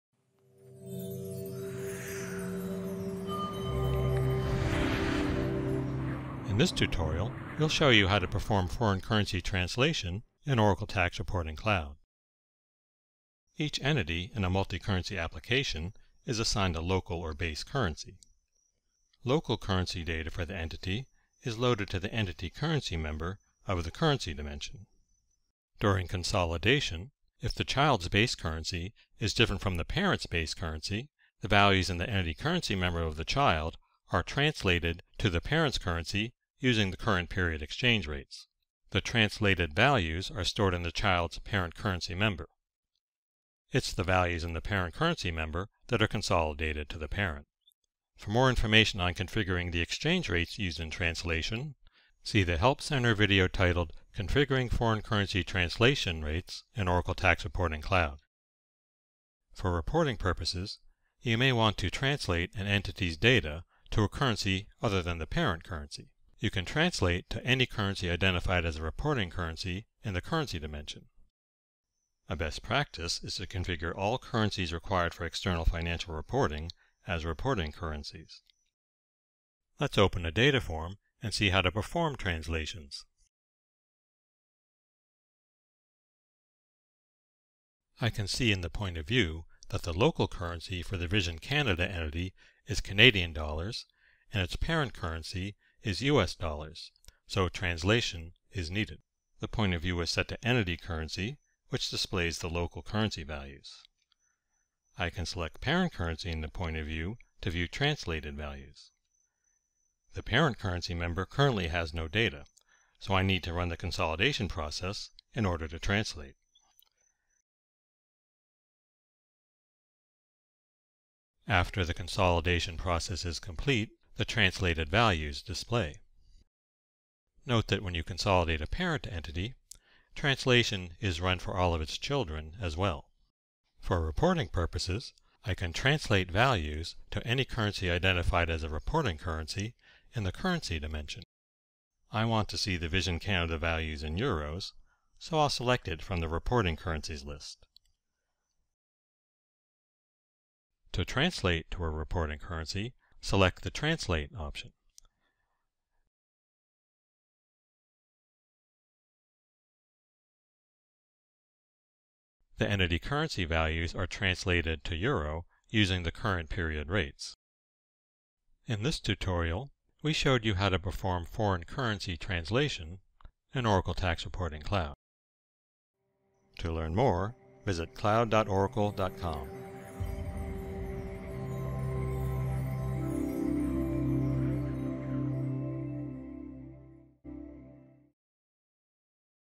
Performing Foreign Currency Translation in Oracle Tax Reporting Cloud
In this tutorial, you learn how to perform foreign currency translation in Oracle Tax Reporting Cloud. You learn how to translate local currency values to the parent currency and to reporting currencies.

Tax Reporting Cloud is now Tax Reporting, a business process in Oracle EPM Cloud.